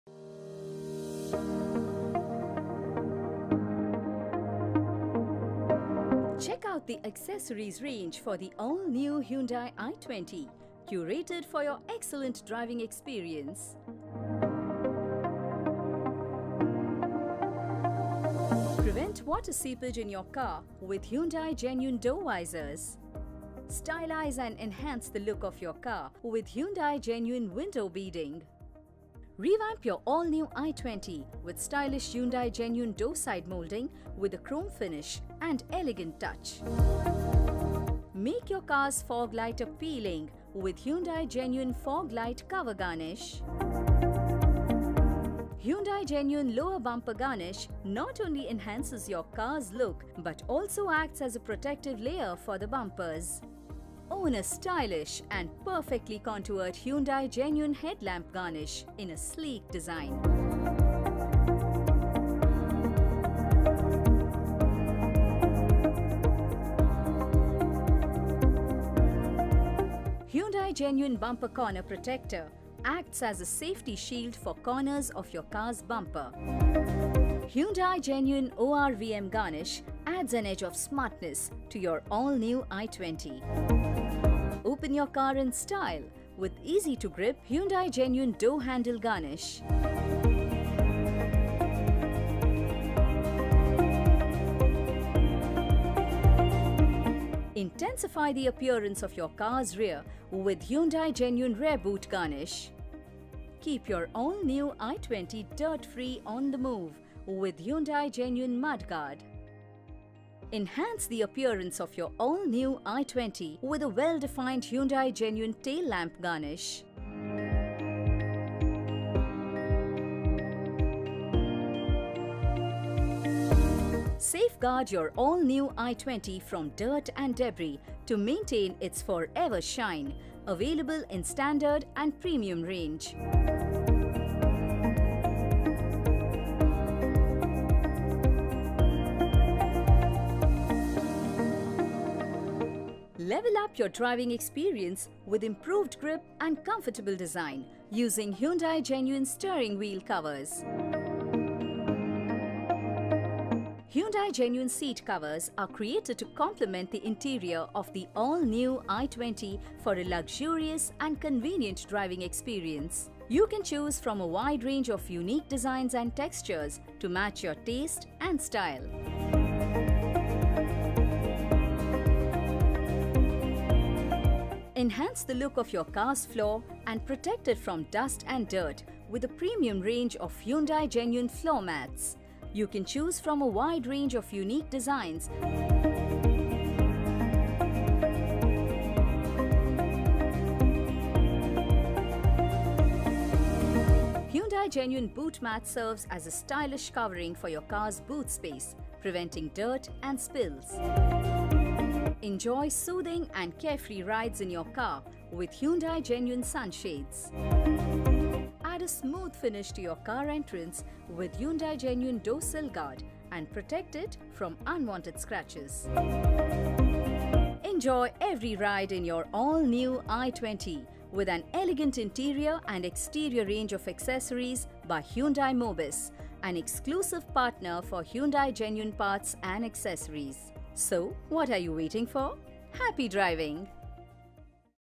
Hyundai Mobis | All-new i20 Accessories
Hit the highway with powerful exteriors combined with elegant interiors.
Presenting all-new i20 with Hyundai Genuine Accessories.